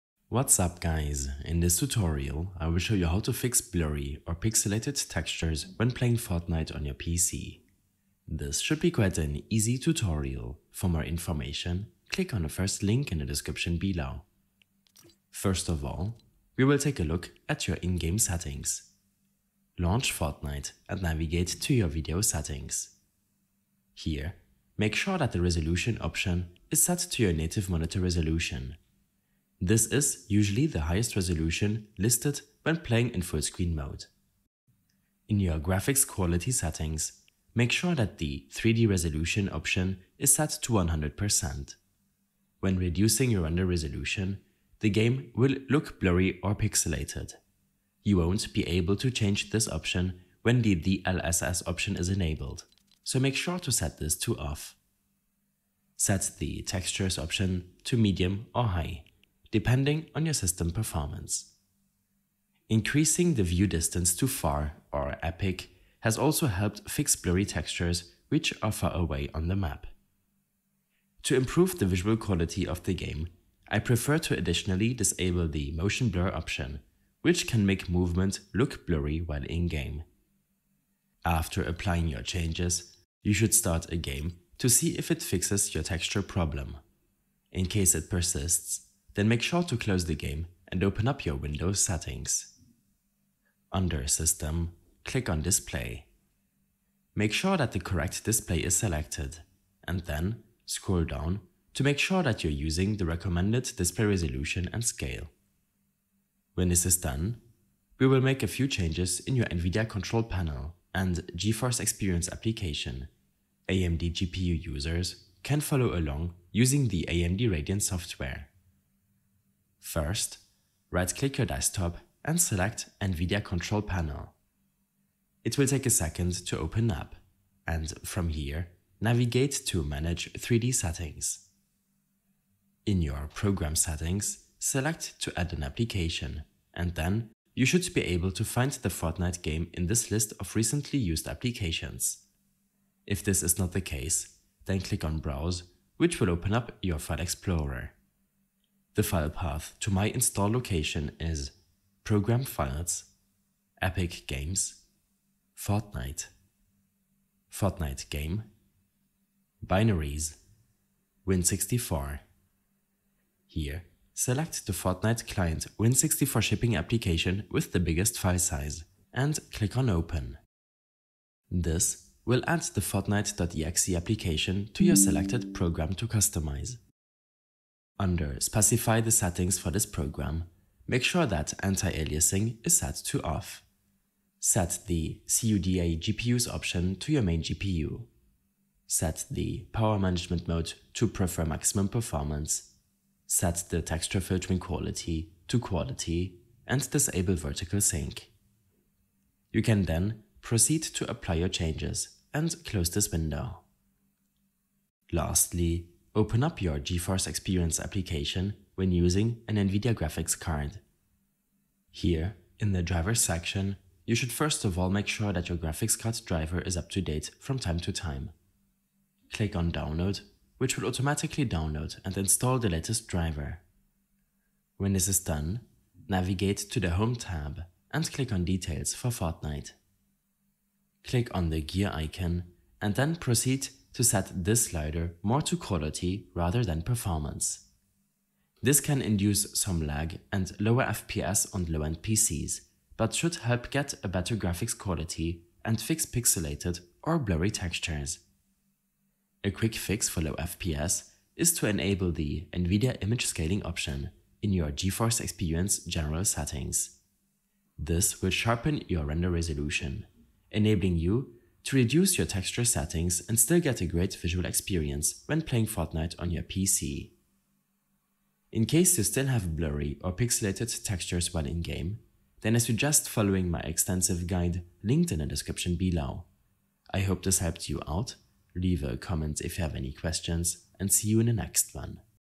How To Fix Textures Blurry & Pixelated in Fortnite
A short tutorial on how to fix blurry or pixelated textures when playing Fortnite on your PC.

Timestamps:
0:00 - Introduction
0:15 - Fortnite Video Settings
1:27 - Windows Display Settings
1:46 - NVIDIA Control Panel
3:25 - Update Graphics Driver
3:46 - Fortnite Quality Settings
4:09 - Enable NVIDIA Image Scaling